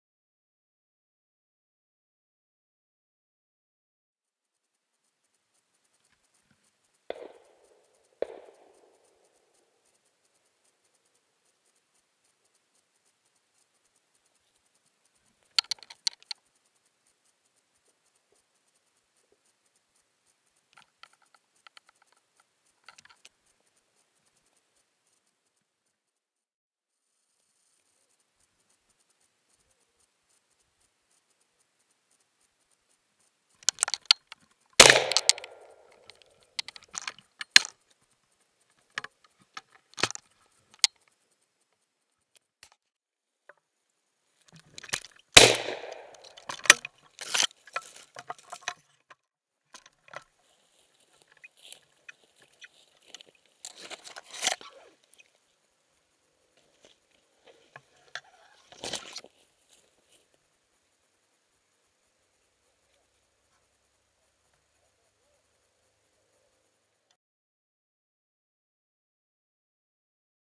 Chasse grive à la remontée
Au travers de ces images vous pourrez vous rendre compte des qualités de la Contour HD avec une faible luminosité. La caméra embarquée Contour HD réalise des vidéo au format 1080p dit aussi de « full HD » (1920x1080)